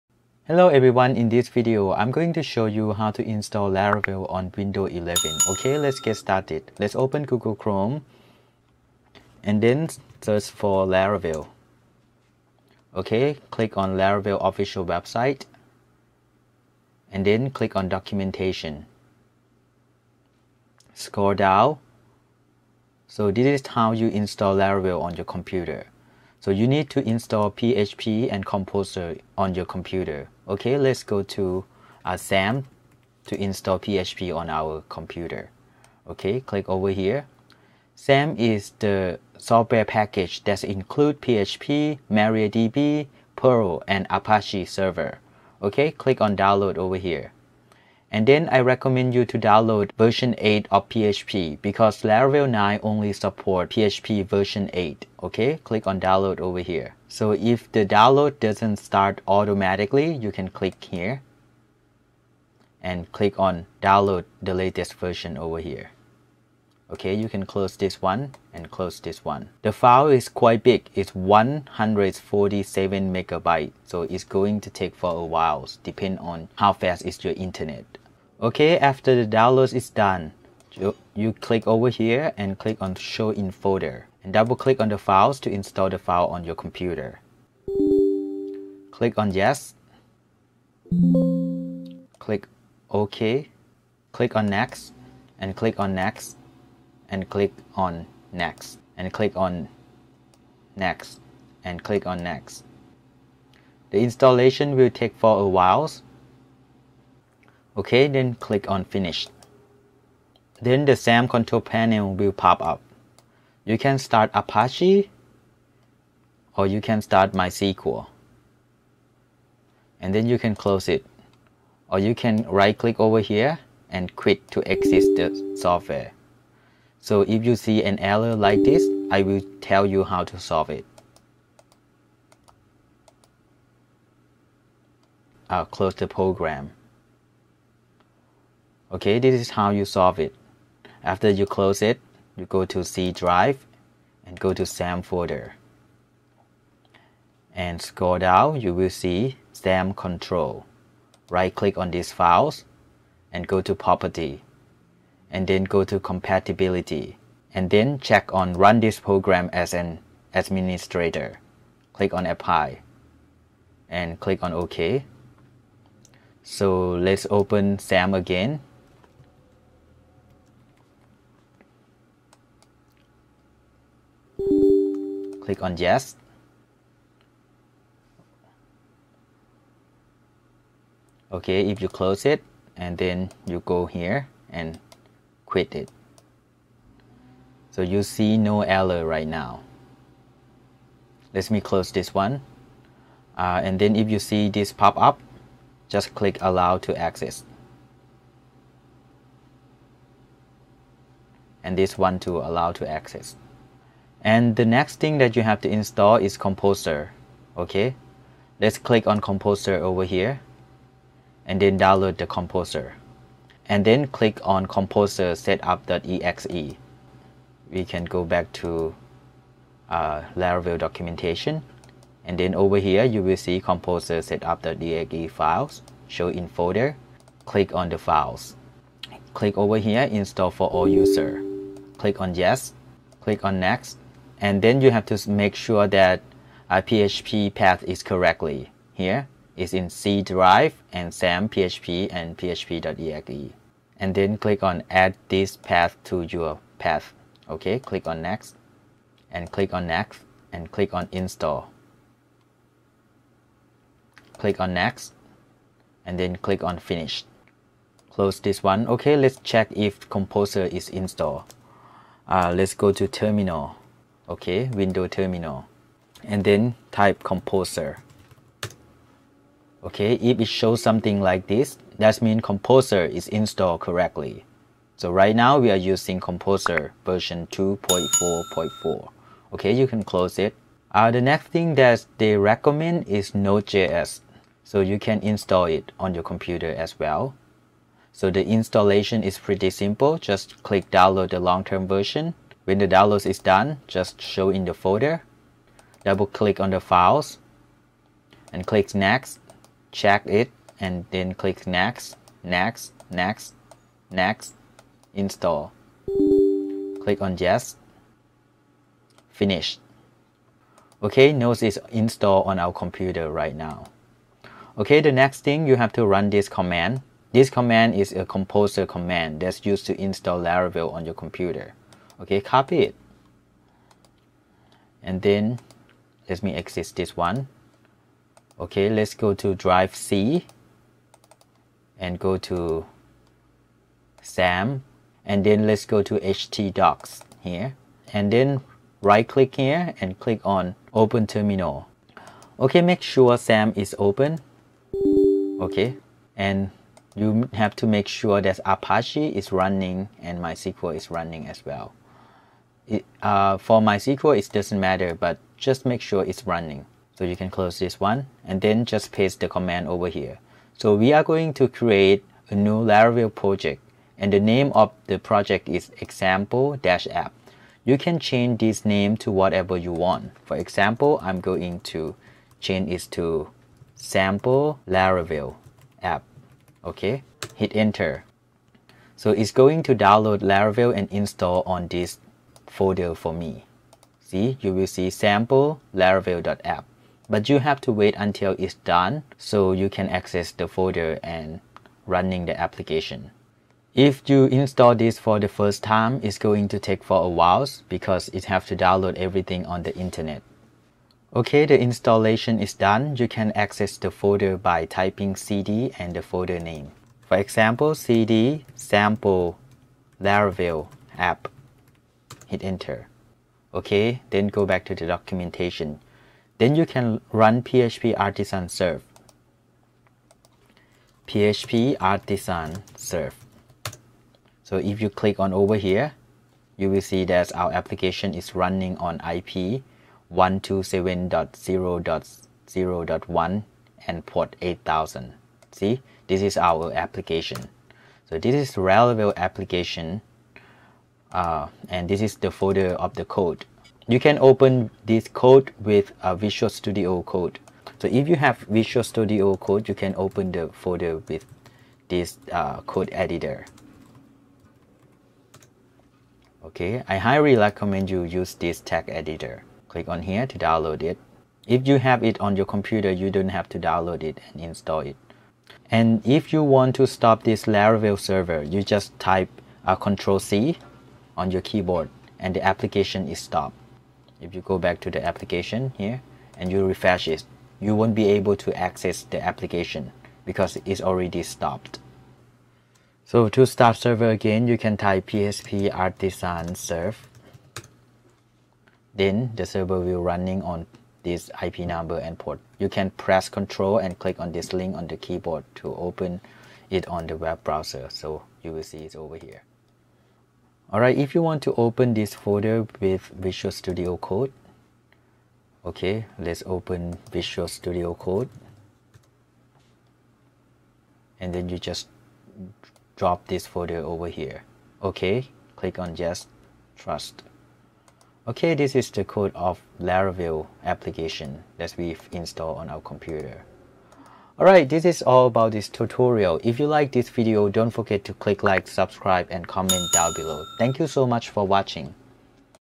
How to Install Laravel on Windows 11 for Beginners
This video shows how to install Laravel 9 on Windows 11 by using Xampp and Composer. If you are on Windows 11, then this tutorial is for you.

First, you need to install XAMP, Composer, and Git bash.